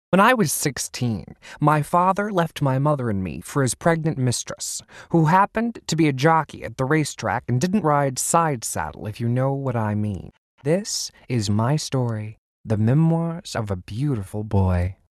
The Memoirs of a Beautfiul Boy Book Trailer from author Robert Leleux
A short book trailer for author Robert Leleux's memoir, The Memoirs of a Beautiful Boy.